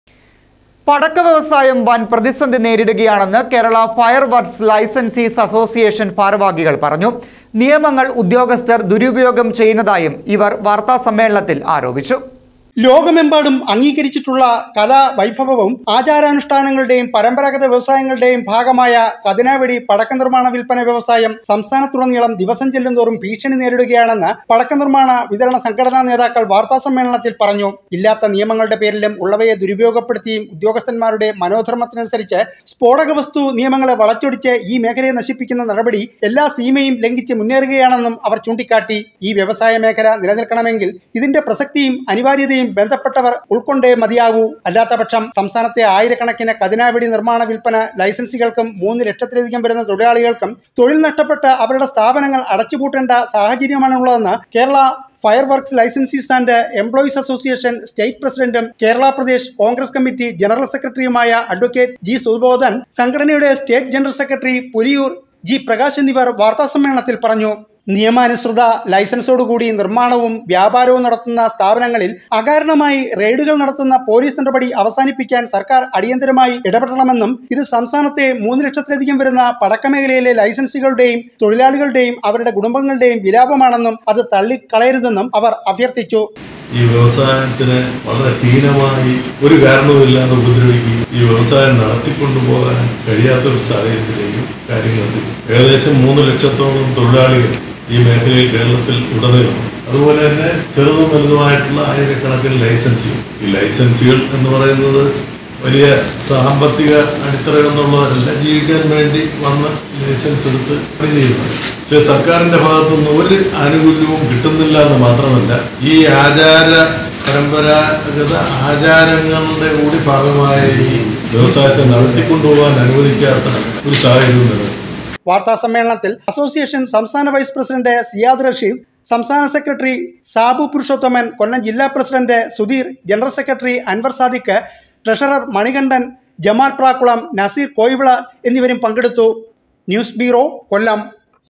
നിയമങ്ങള്‍ ഉദ്യോഗസ്ഥര്‍ ദുരുപയോഗം ചെയ്യുന്നതായും അവര്‍ ആരോപിച്ചു
പടക്ക വ്യവസായം വന്‍ പ്രതിസന്ധി നേരിടുകയാണെന്ന് കേരളാ ഫയര്‍ വര്‍ക്ക് സ് ലൈസന്‍സീസ് അസോസിയേഷന്‍ ഭാരവാഹികള്‍ പറഞ്ഞു. നിയമങ്ങള്‍ ഉദ്യോഗസ്ഥര്‍ ദുരുപയോഗം ചെയ്യുന്നതായും അവര്‍ വാര്‍ത്താ സമ്മേളനത്തില്‍ ആരോപിച്ചു.